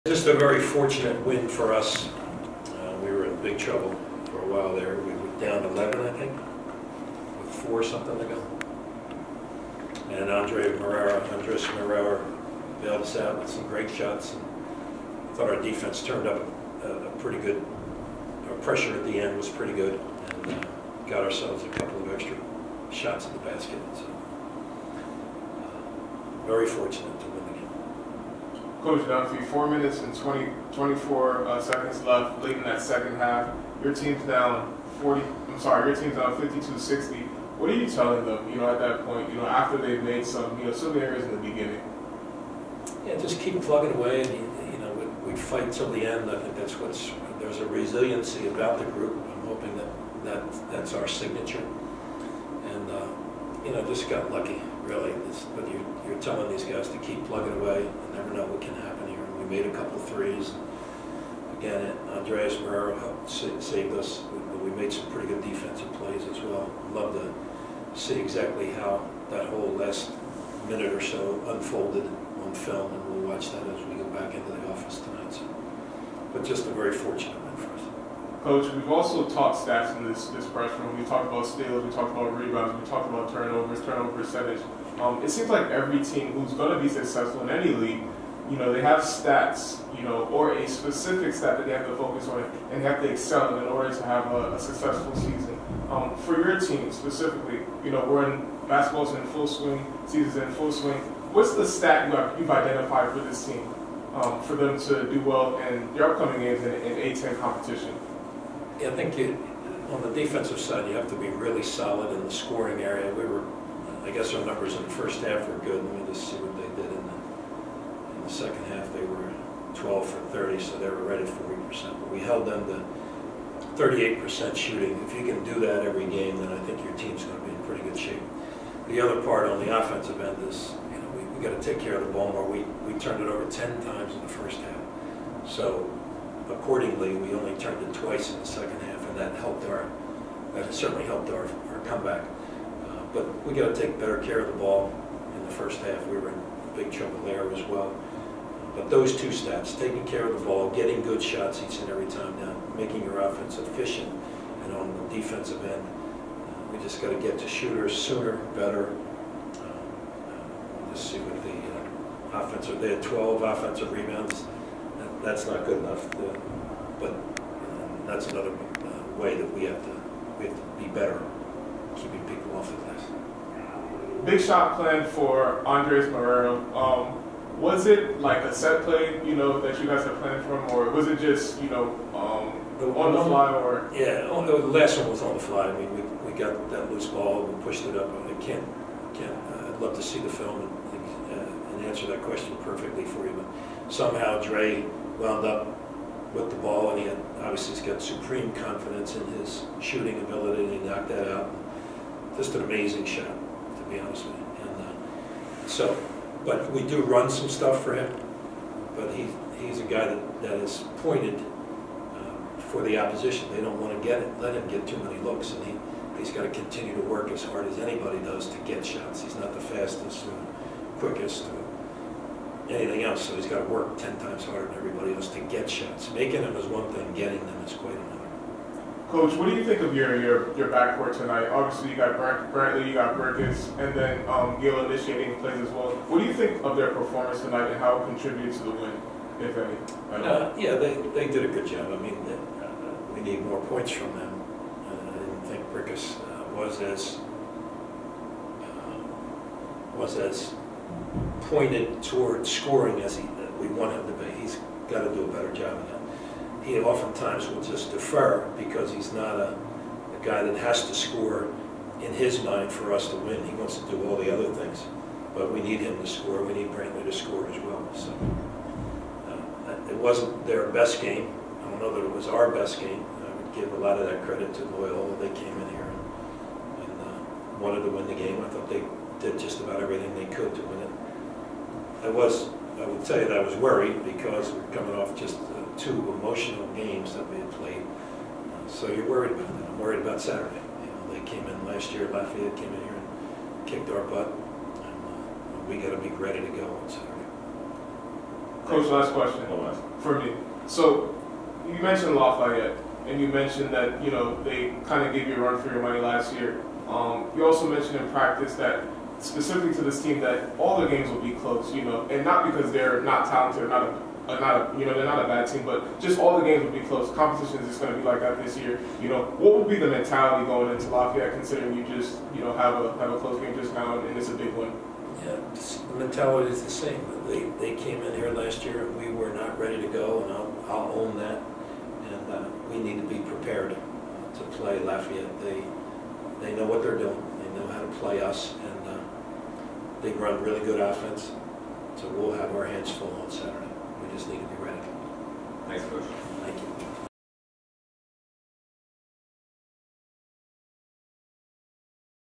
IBX Press Conference: La Salle MBB vs. Loyola MD
La Salle men's basketball head coach Fran Dunphy takes questions from the media following the team's 62-61 win over Loyola MD.